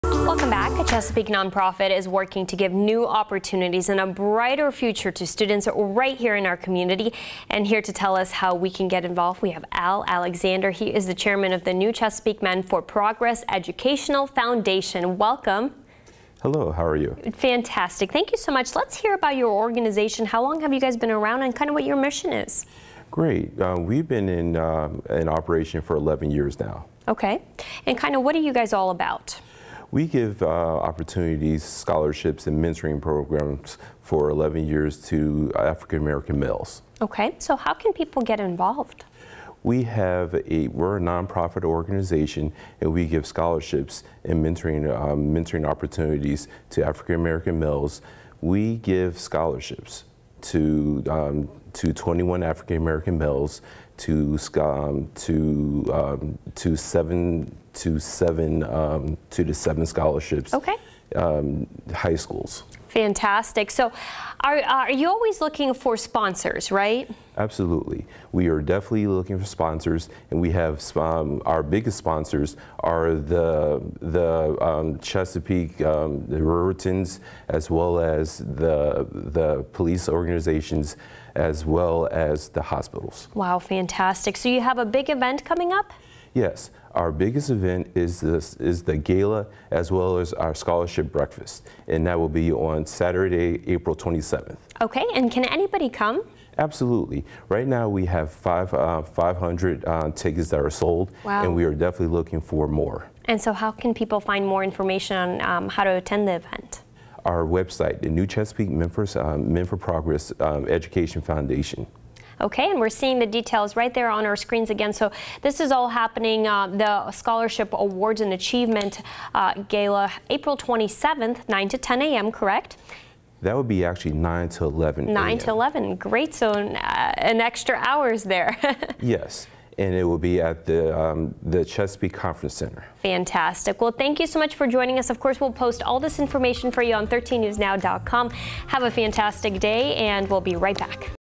Noon interview: Black male achievement and scholarship awards
A Chesapeake nonprofit is working to give new opportunities to students right here in our community.